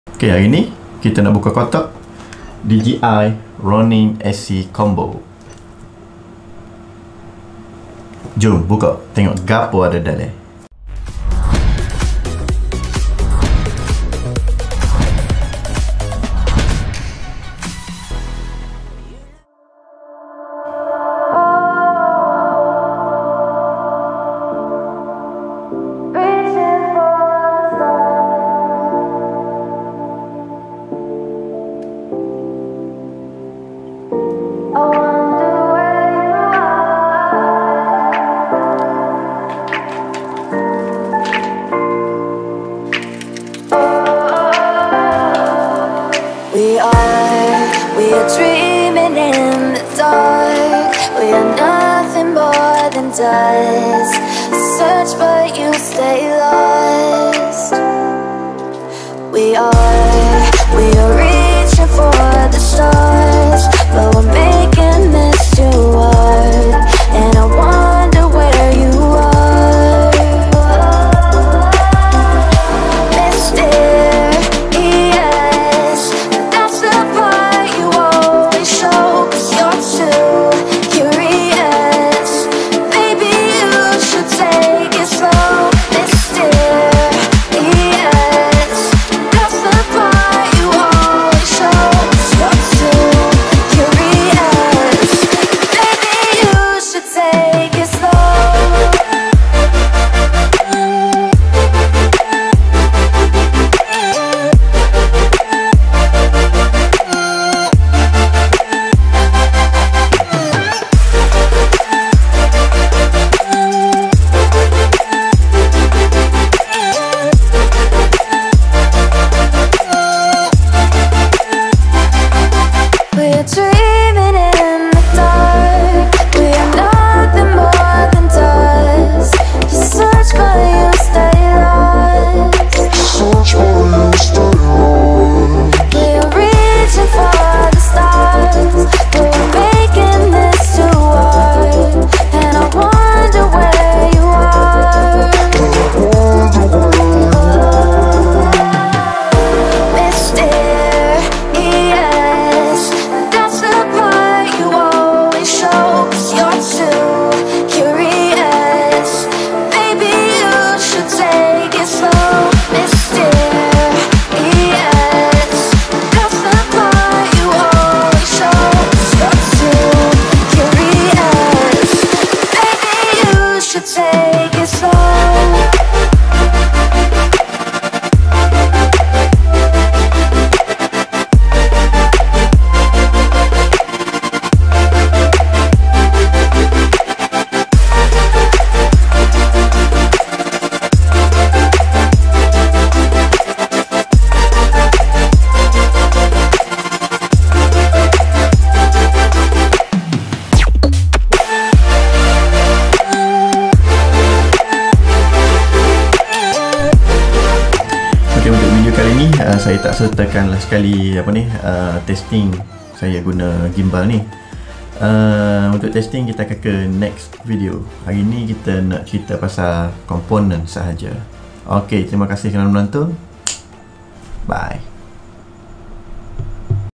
DJi RONiN SC - Buka Kotak @ Open Box
Video kalini aku just buat utk openbox DJI RoninSc .. di mana gimbal ni aku baru beli semata2 utk buat job wedding.. alang2 dah ada kat tgn ni aku buat video unboxing ni.. utk korang yg nk tgok komponen apa kat dalam kotak ronin sc ni,,

LAYAN...

Jgn lupa : SUBSSCIRE ya.. tq kengkawan.. :)